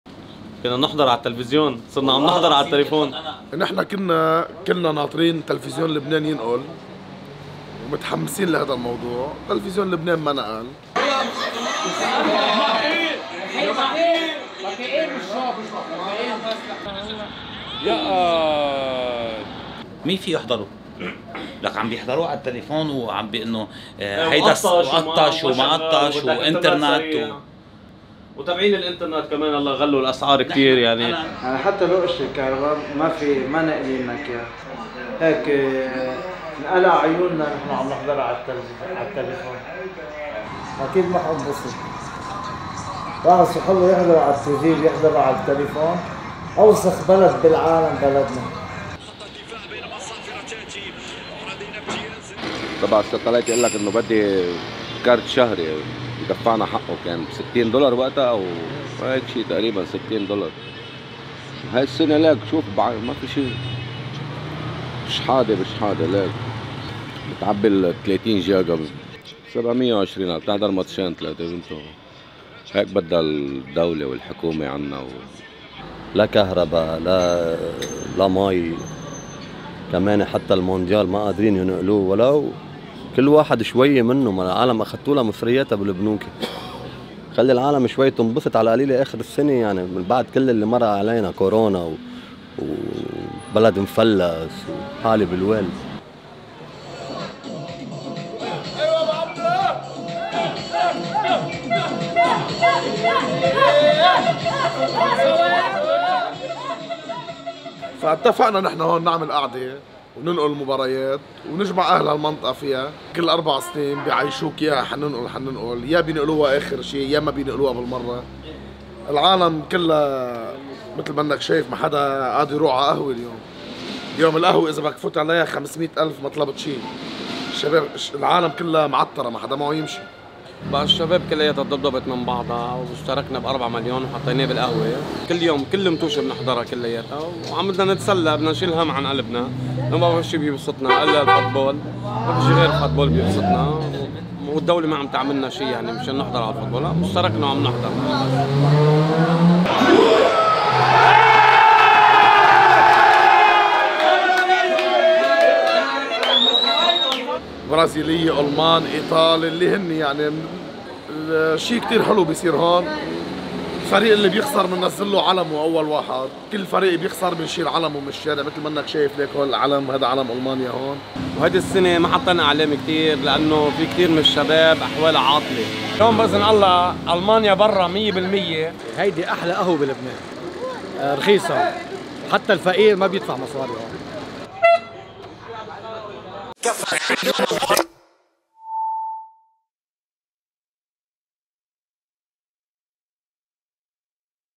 مونديال في زمن الفراغ
تقاعست الدولة اللبنانية وعجزت عن تأمين بثّ مباريات المونديال مجاناً للمُقيمين، فتركتهم رهينةً لاحتكار شركة «سما» واشتراكاتها الخاصة.